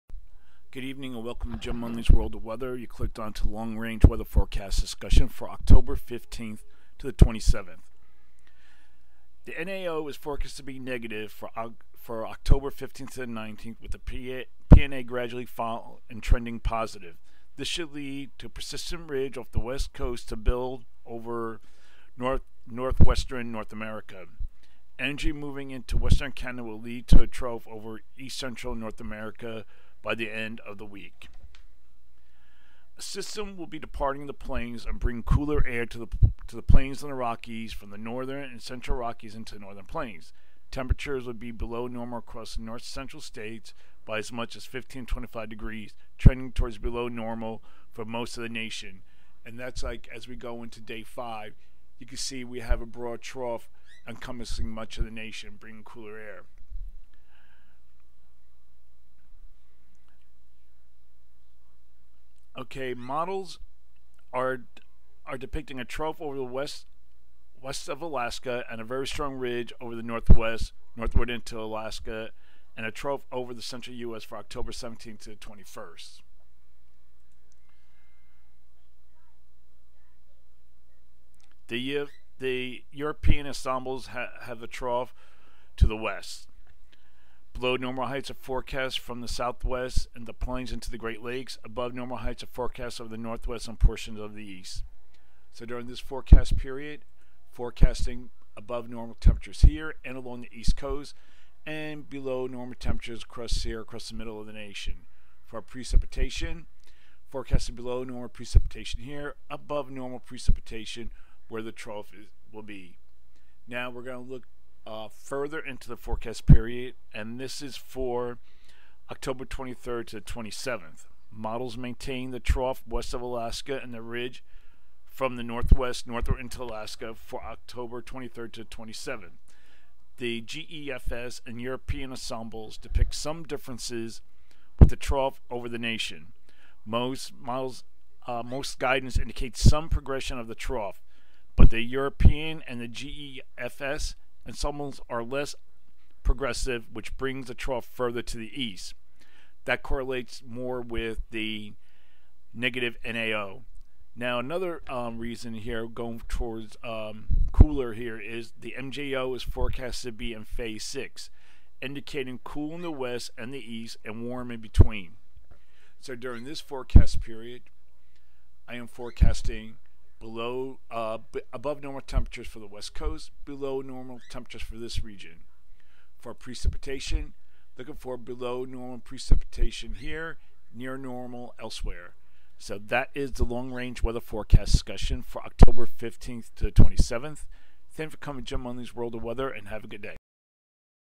Long Range Weather Forecast Discussion October 15 27
Cold and snow for parts of the Rockies and into the Plains.  Colder air is expected to spread east.